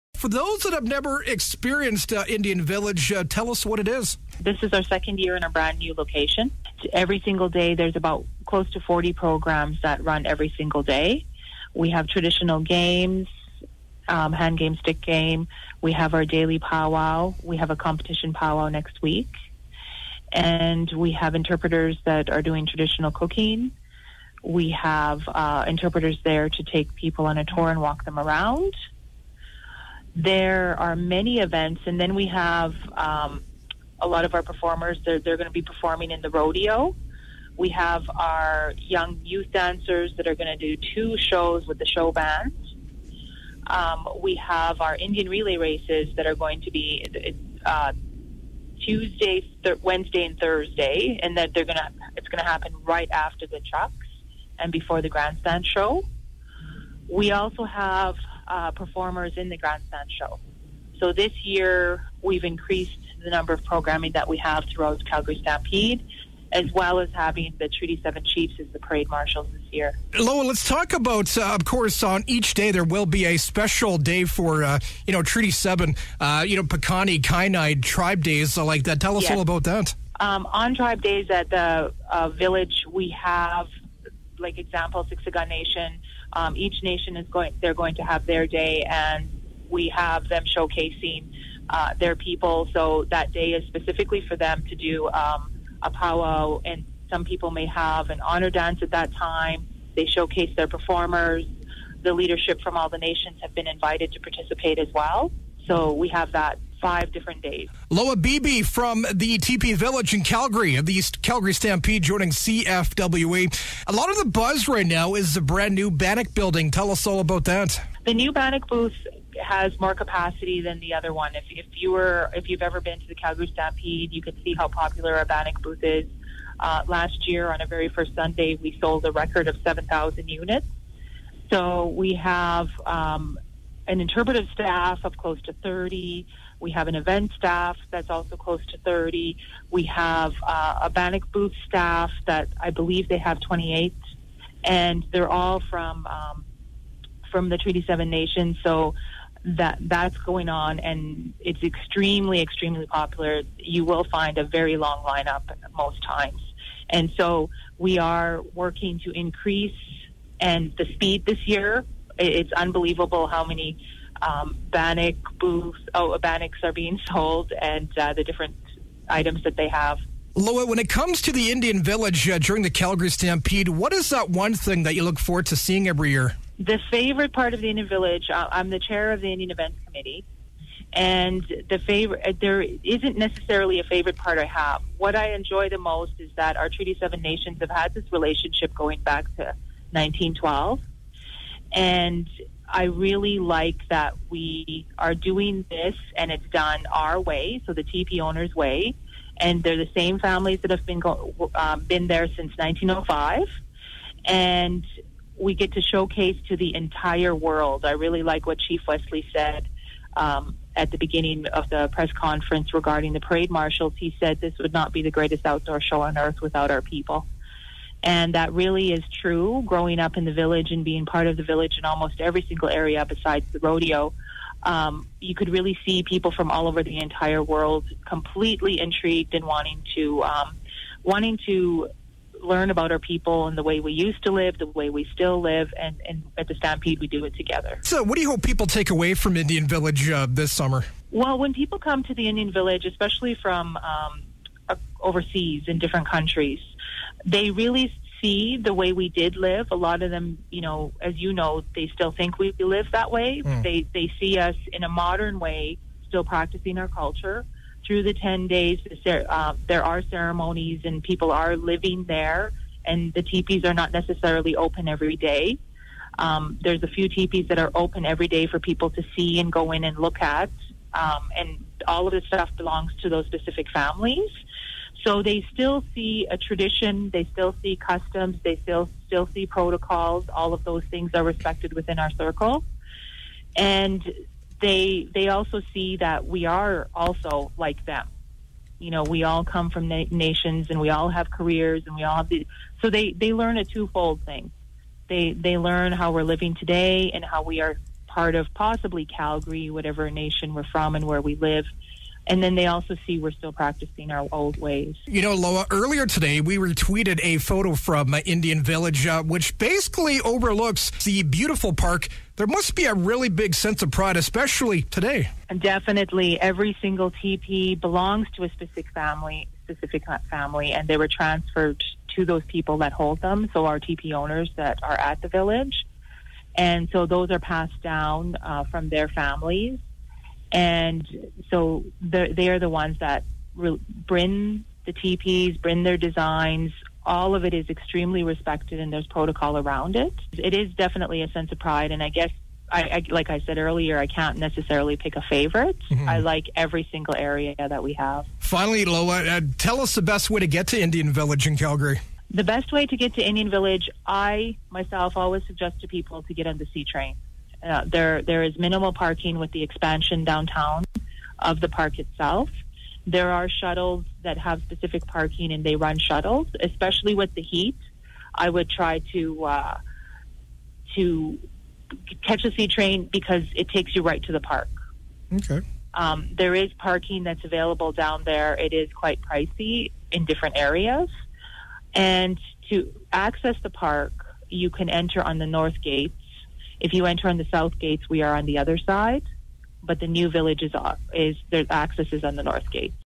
Indian Village @ Calgary Stampede
Lowa Beebe, chair of the Indian Events @ Calgary Stampede joined CFWE to promote Indian Village.